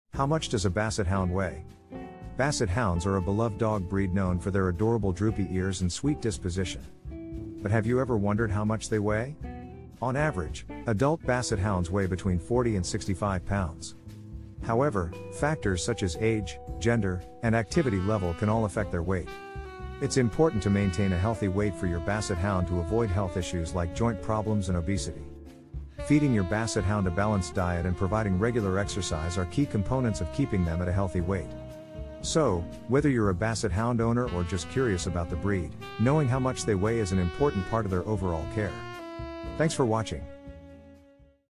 How Much Does A Basset Hound Weigh?
How Much Does A Basset Hound Weigh? - In this short video, we answer the commonly asked question, "How much does a Basset Hound weigh?" If you're considering adopting a Basset Hound or just curious about their size, this video is for you!